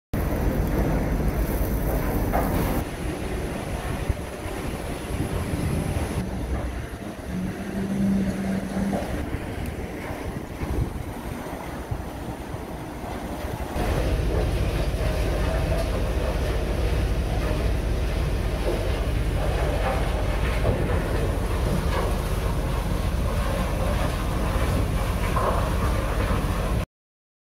Dosing hopper with inclined conveyor - ButeX
A high capacity hopper, a dosing feeder belt and an inclined conveyor designed in a single machine.